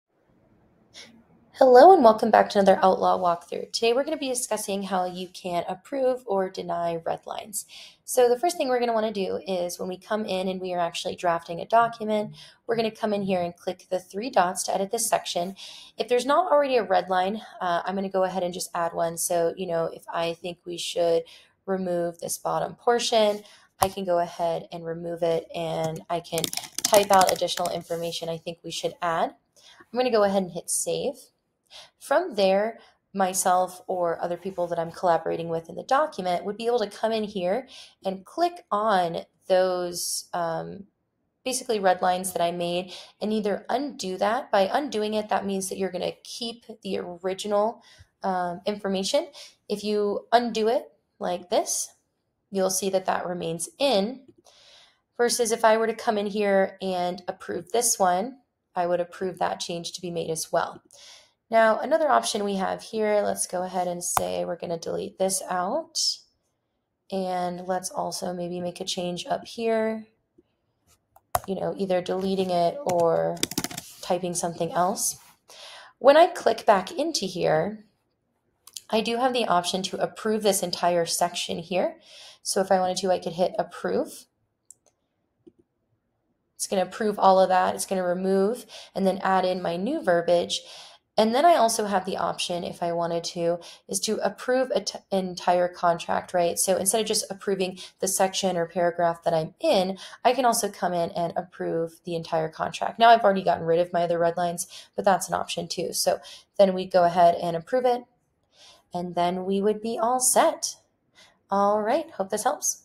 Outlaw: How to Approve or Deny Redlining
In this Outlaw walkthrough, Optimizer Jordan demonstrates how to add, remove, and approve changes within a document, for better collaboration and clarity. She'll guide you through the steps of editing sections, approving specific changes, and even approving entire contracts.